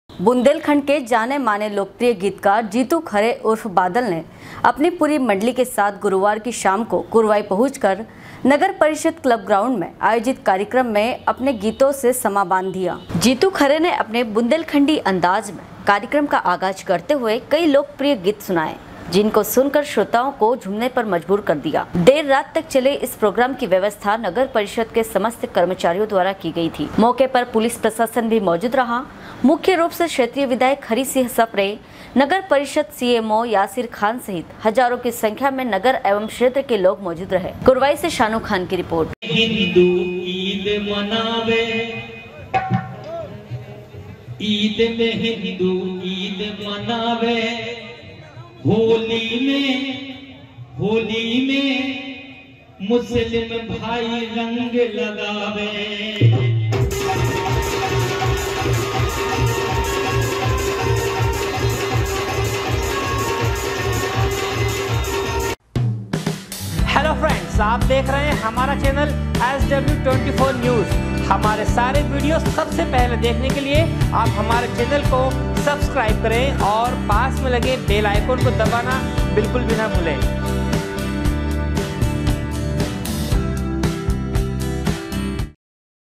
बुंदेलखंड के लोकप्रिय गीतकार जित्तू खरे उर्फ बादल ने अपनी पूरी मंडली के साथ गुरुवार की शाम को कुरवाई
बुंदेलखंड के जाने-माने लोकप्रिय गीतकार जित्तू खरे उर्फ बादल ने अपनी पूरी मंडली के साथ गुरुवार की शाम को कुरवाई पहुंच कर नगर परिषद क्लब ग्राउंड मैं आयोजित कार्यक्रम मे अपने गीतो से समा बांध दिया।
कुरवाई से शानू खान की रिपोर्ट।